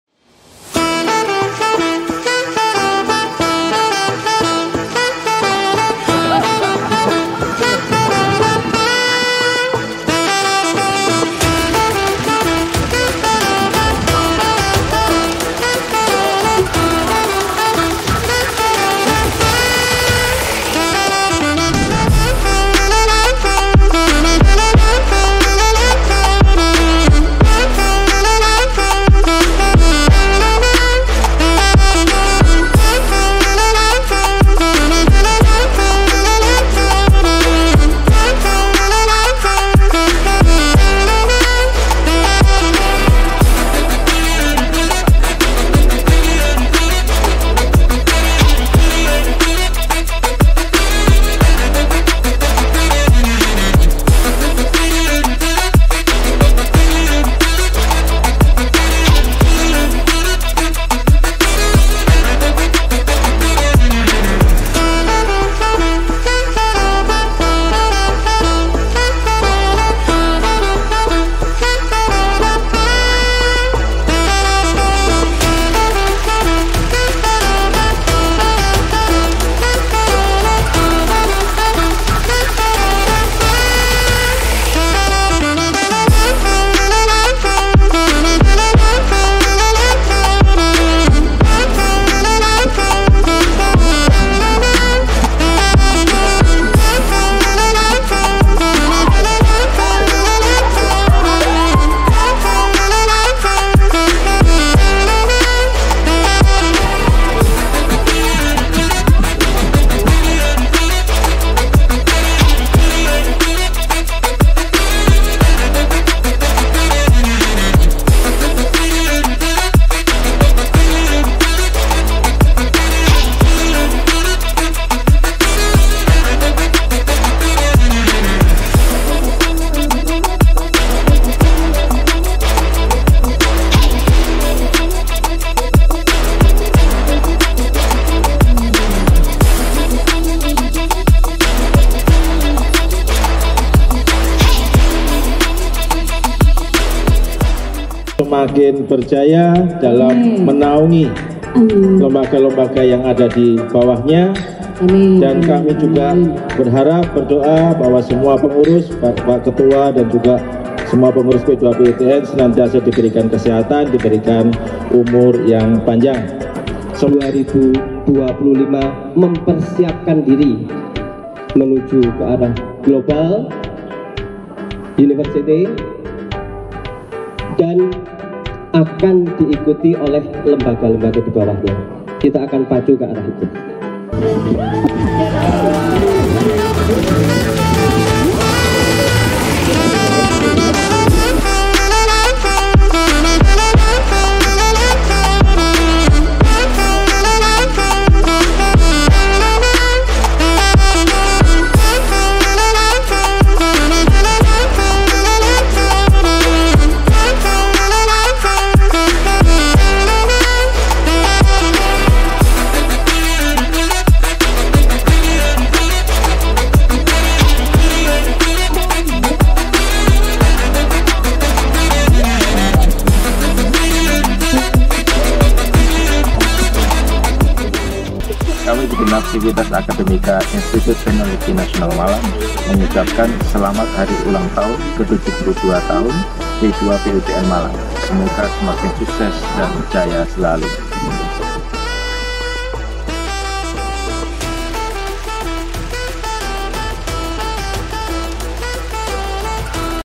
Kemeriahan Fun Walk P2PUTN Malang 2023
Meriahnya fun walk memperingati hari jadi ke 72 tahun Perkumpulan Pengelola Pendidikan Umum dan Teknologi Nasional (P2PUTN) Malang, pada pekan kemarin.

Bertujuan menguatkan karakter untuk melahirkan generasi hebat. Fun work diikuti oleh ribuan peserta gabungan dari institusi dibawah naungan P2PUTN. Yakni SMP Nasional, SMA Nasional, SMK Nasional, dan ITN Malang. Serta alumni dan masyarakat sekitar kampus.

“Lembaga di bawah P2PUTN tidak lagi terkotak-kotak, namun menjadi satu dalam visi misi pendidikan. Ayo bersinergi membangun Indonesia.” (Ketua P2PUTN Malang, Ir. Kartiko Ardi Widodo., MT.)